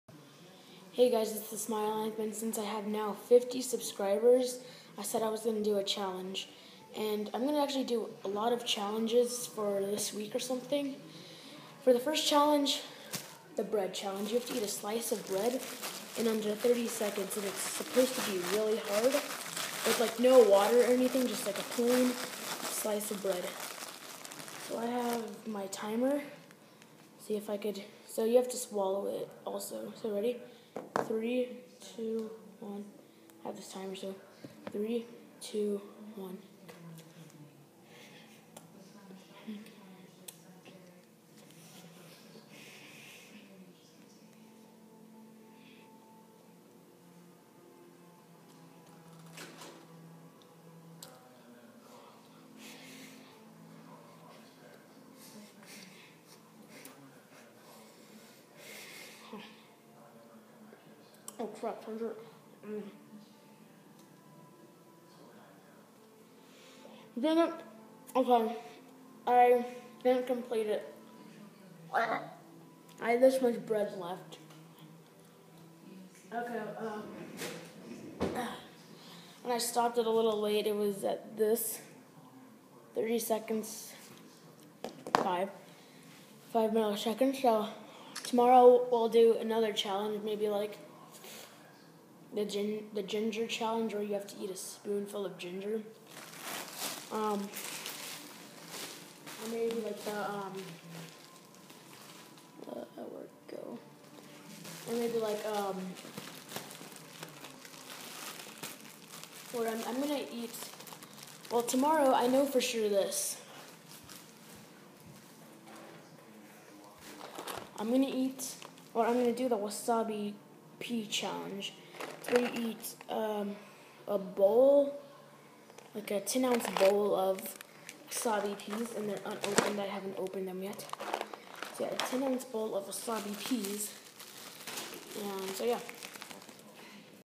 50 subscriber special bead challenge
You have to eat a slice of bread in 30 seconds and its hard because u u can't lick your lips and it makes your mouth dry
Fan mail is tomarrow